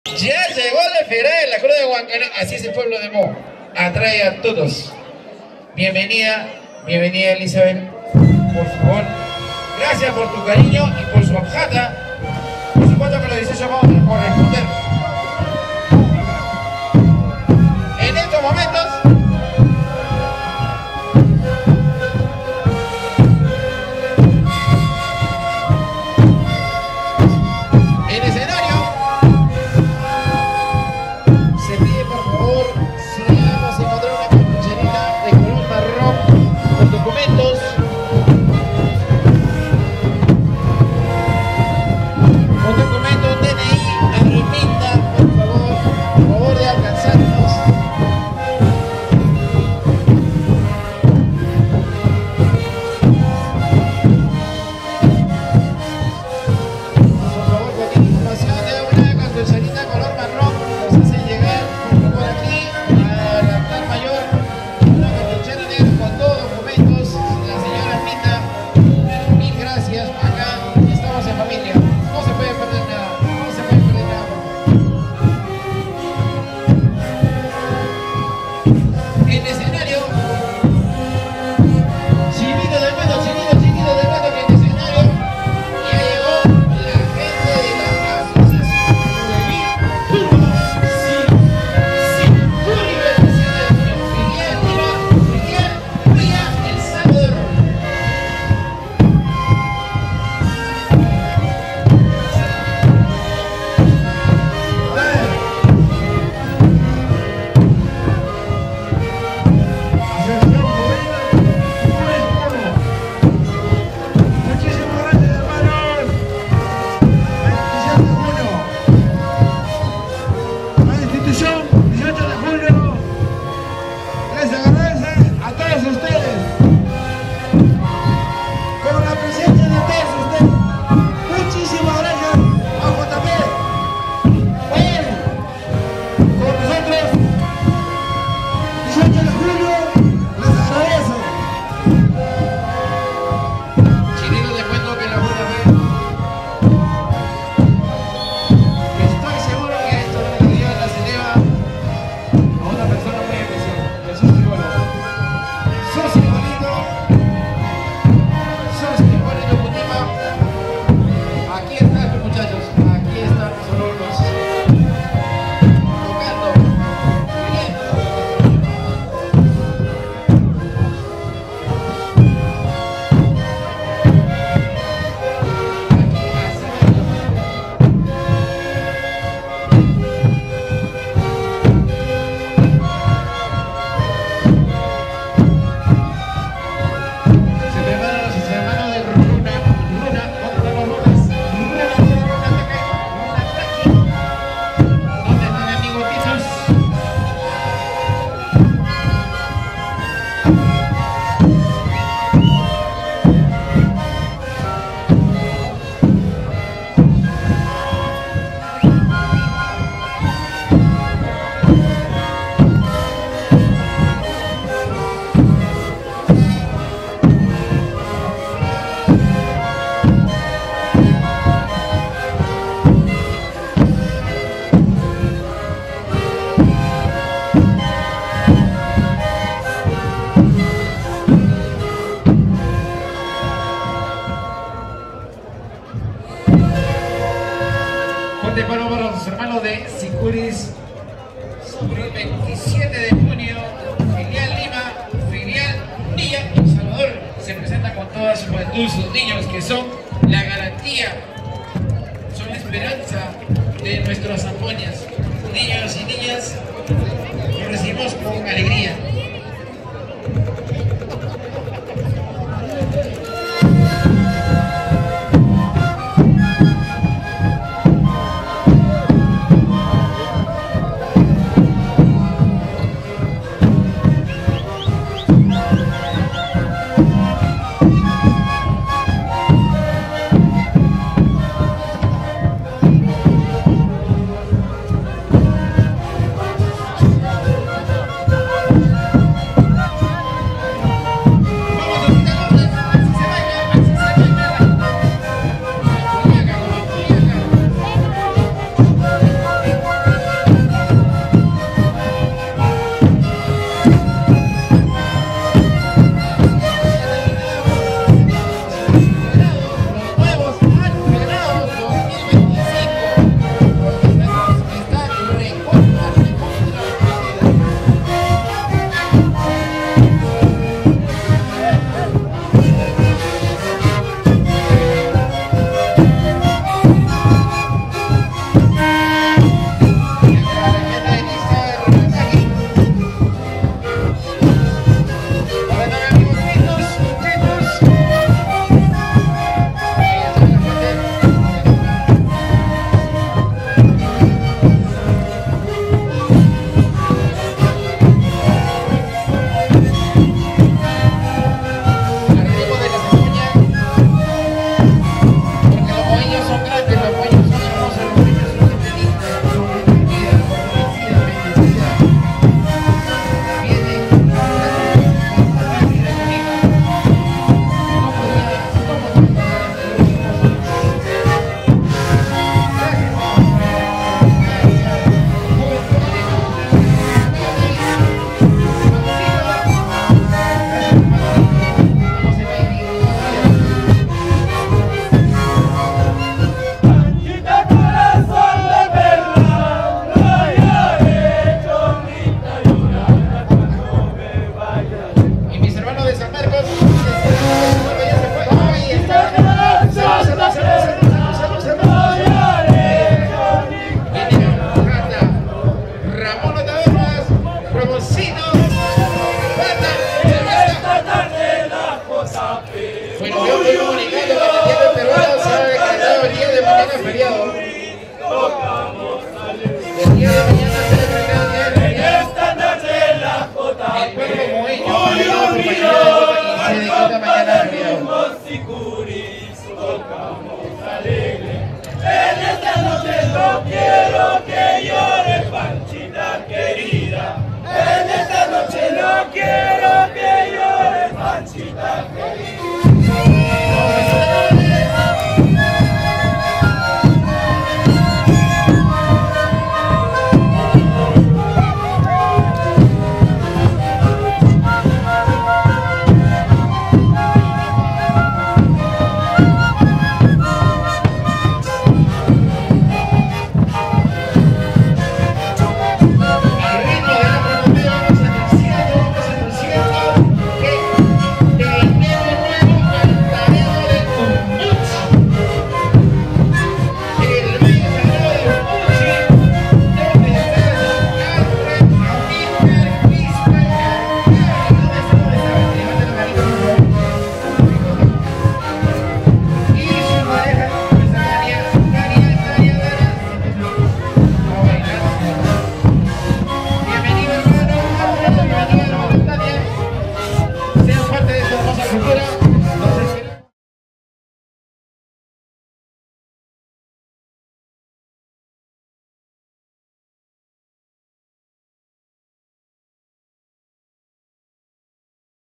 𝐂𝐑𝐔𝐙 𝐃𝐄 𝐌𝐀𝐘𝐎 𝐃𝐄 𝐌𝐎𝐇𝐎 2024: 𝑨𝑱𝑷 - 𝑪𝒐𝒏𝒋𝒖𝒏𝒕𝒐 𝒅𝒆 𝑺𝒊𝒄𝒖𝒓𝒊𝒔 27 𝒅𝒆 𝑱𝒖𝒏𝒊𝒐 - 𝑭𝒊𝒍𝒊𝒂𝒍 𝑳𝒊𝒎𝒂 𝒚 𝑽𝒊𝒍𝒍𝒂 𝑬𝒍 𝑺𝒂𝒍𝒗𝒂𝒅𝒐𝒓
𝑨𝒔𝒐𝒄𝒊𝒂𝒄𝒊𝒐́𝒏 𝑱𝒖𝒗𝒆𝒏𝒊𝒍 𝑷𝒖𝒏𝒐 - 𝑪𝒐𝒏𝒋𝒖𝒏𝒕𝒐 𝒅𝒆 𝑺𝒊𝒄𝒖𝒓𝒊𝒔 27 𝒅𝒆 𝑱𝒖𝒏𝒊𝒐 - 𝑬𝒍𝒆𝒏𝒄𝒐 𝒅𝒆 𝑫𝒂𝒏𝒛𝒂𝒔 𝑪𝒂𝒏𝒅𝒆𝒍𝒂𝒓𝒊𝒂 𝑯𝒆𝒓𝒓𝒆𝒓𝒂 - 𝑭𝒊𝒍𝒊𝒂𝒍 𝑳𝒊𝒎𝒂 𝒚 𝑭𝒊𝒍𝒊𝒂𝒍 𝑽𝒊𝒍𝒍𝒂 𝑬𝒍 𝑺𝒂𝒍𝒗𝒂𝒅𝒐𝒓
𝐒𝐀𝐍𝐓𝐈́𝐒𝐈𝐌𝐀 𝐂𝐑𝐔𝐙 𝐃𝐄 𝐌𝐀𝐘𝐎 𝐃𝐄 𝐌𝐎𝐇𝐎.
𝑨𝒍𝒇𝒆𝒓𝒂𝒅𝒐𝒔: Conjunto de Sikuris 18 de Julio y Elenco de Danzas "Por el Respeto a la Vida"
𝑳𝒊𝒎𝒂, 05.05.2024